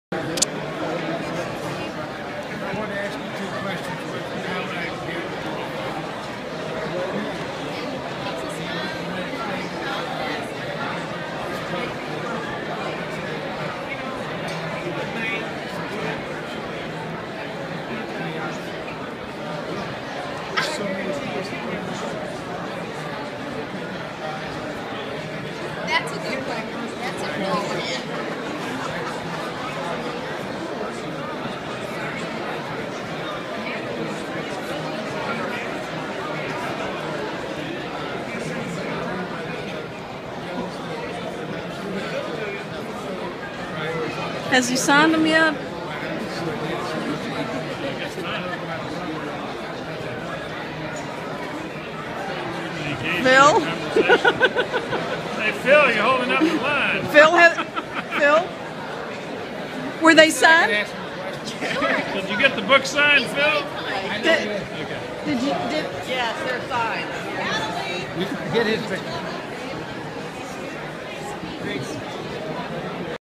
Ben Carson 10 4
Phil grills Dr. Carson.  You'll have to make up the dialogue but Phil asked him questions about statements he recently made.  What a great man and very good speaker.